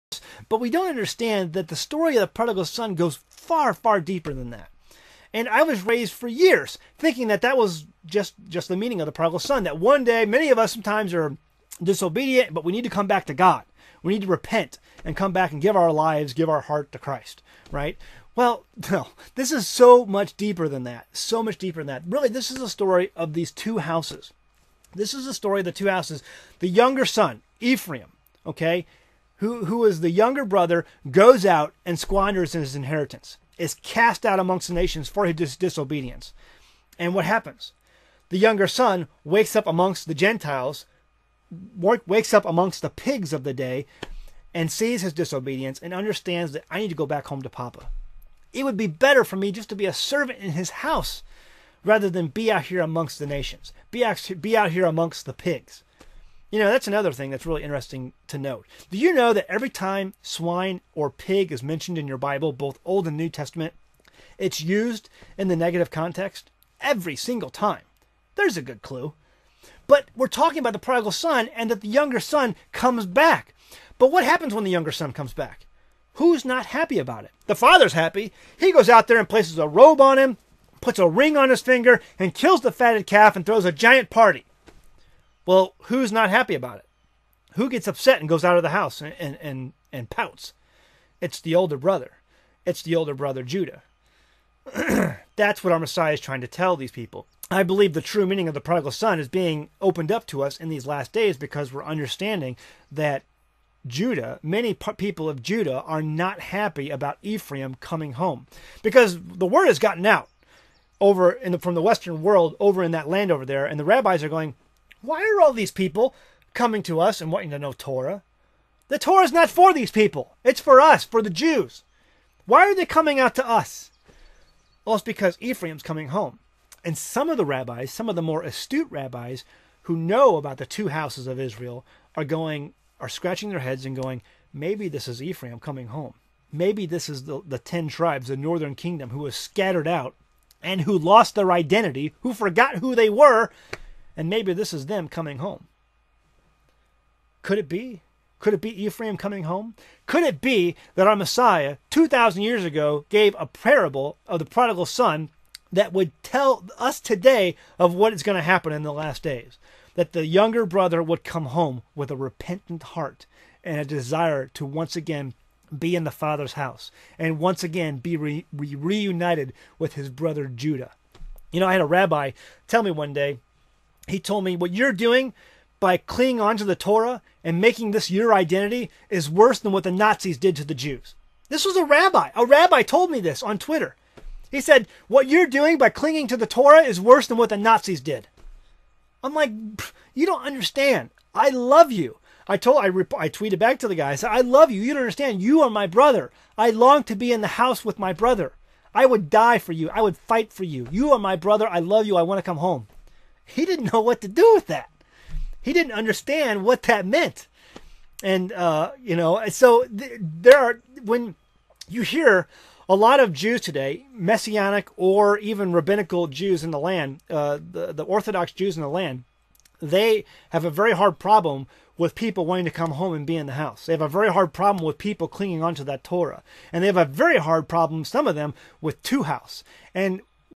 Parable of the Prodigal Son by Zach Bauer
Zach shares a prophetic message found within the Parable of the Prodigal Son.